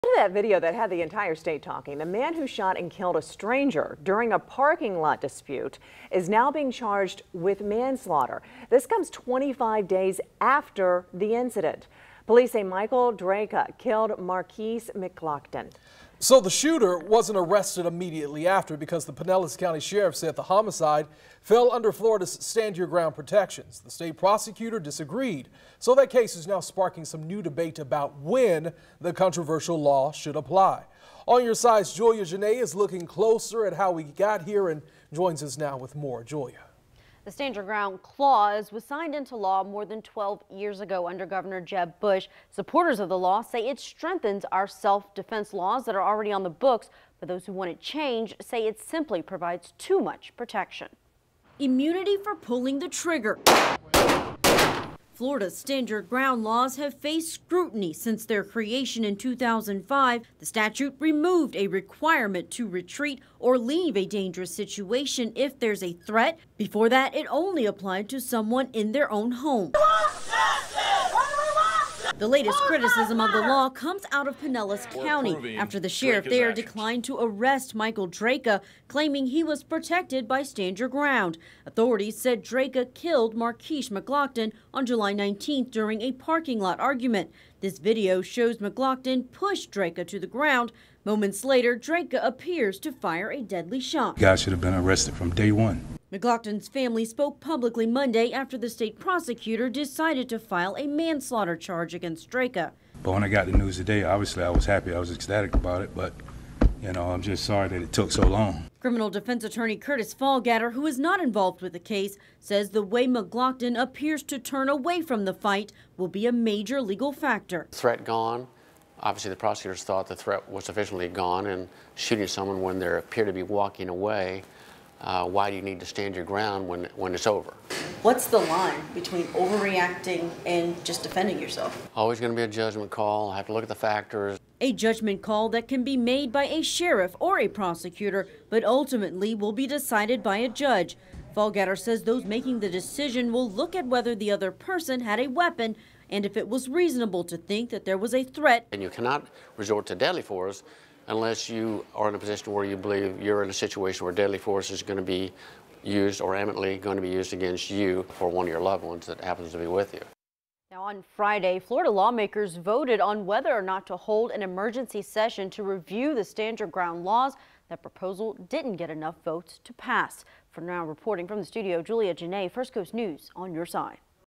Do you know your rights? FCN takes a closer look at Florida's Stand Your Ground laws
Florida's Stand Your Ground laws have faced scrutiny since their creation in 2005.